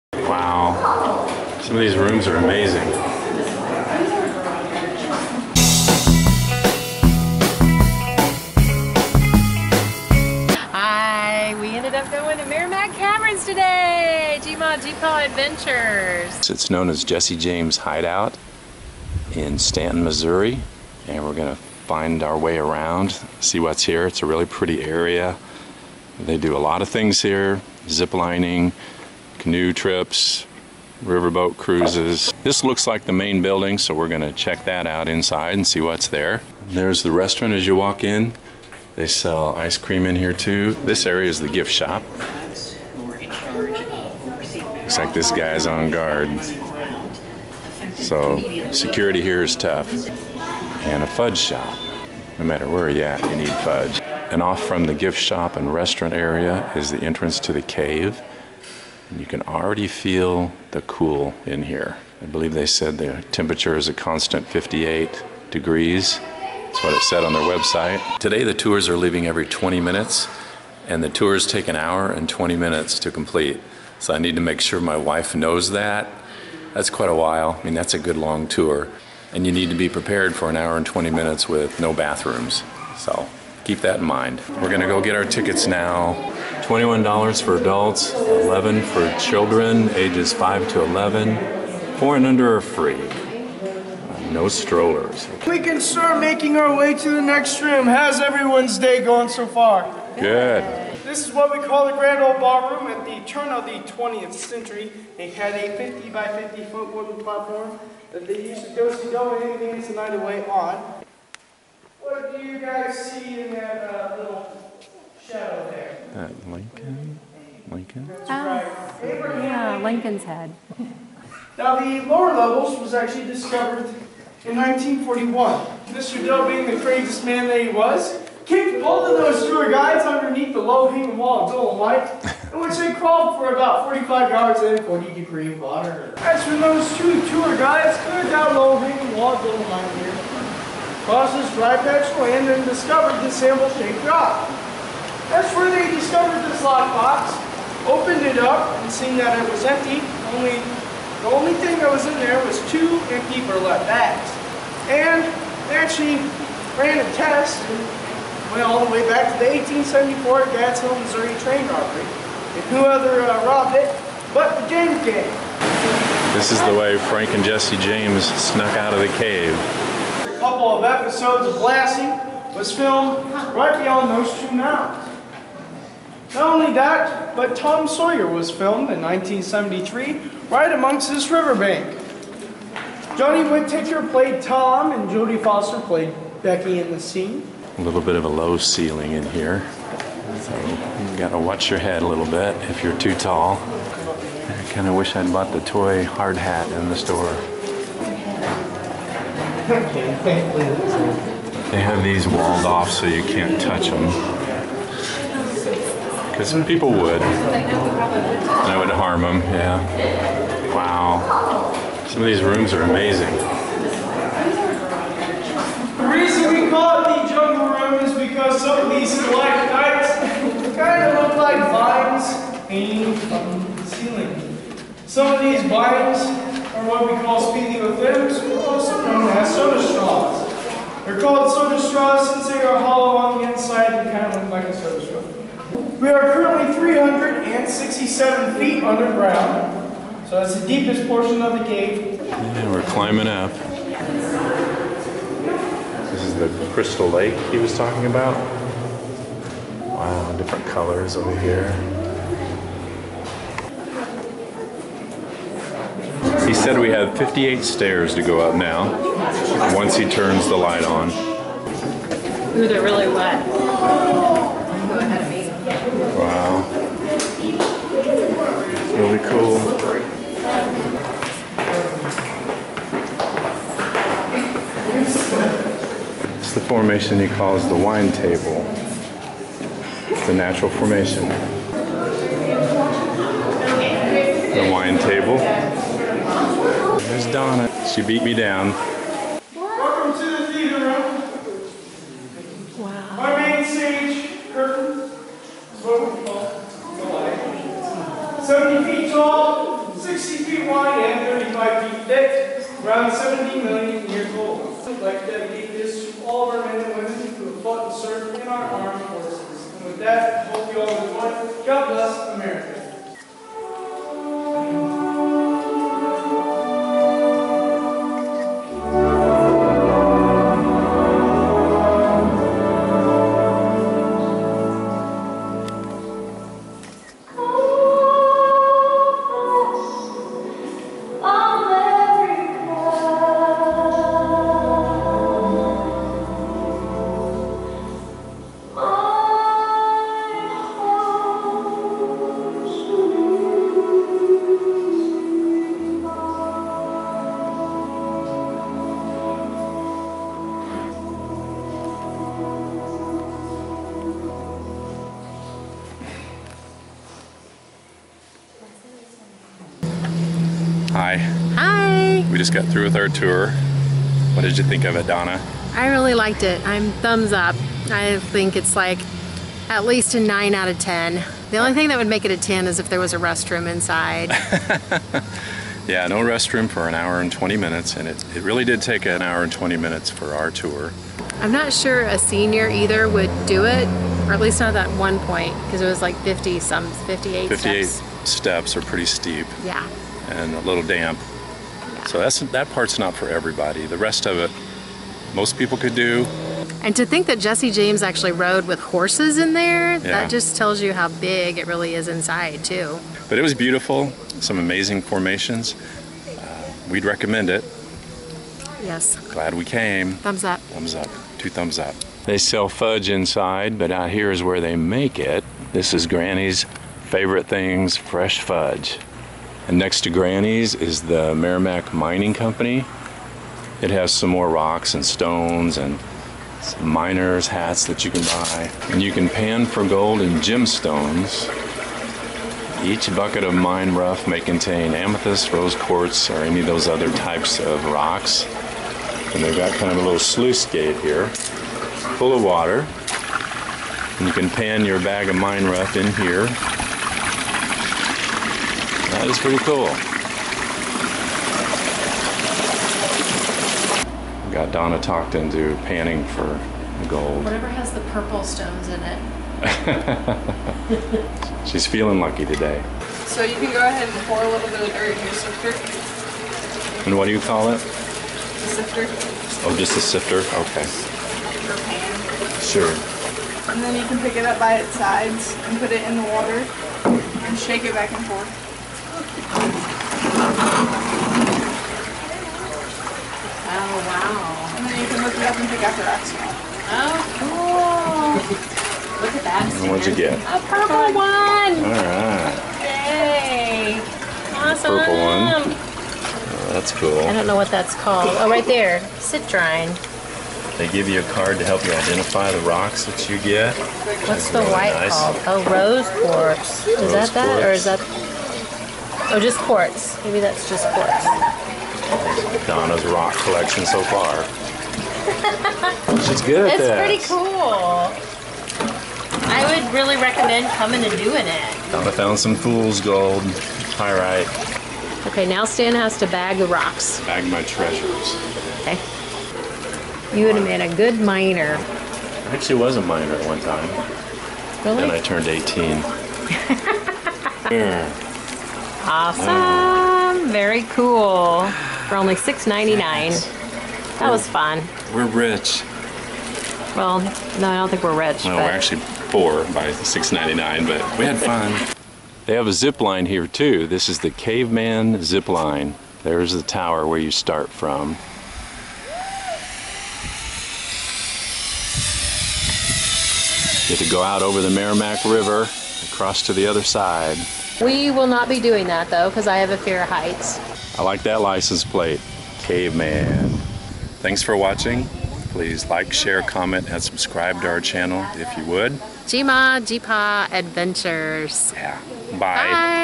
Meramec Caverns Tour On Route 66 - Jesse James Hideout - America's Cave Stanton Missouri Ozarks
Meramec Caverns is a classic Route 66 tourist attraction located in Stanton, Missouri. We hadn't been there since we were kids, so our childhood memories of it drew us to revisit it! Meramec Caverns, referred to as Jesse James Hideout and America's Cave, offers a 1 hour and 20 minute walking tour of the huge limestone cave system. There are many other thincgs to do on the property besides the cave tour, making this a great family-friendly vacation destination!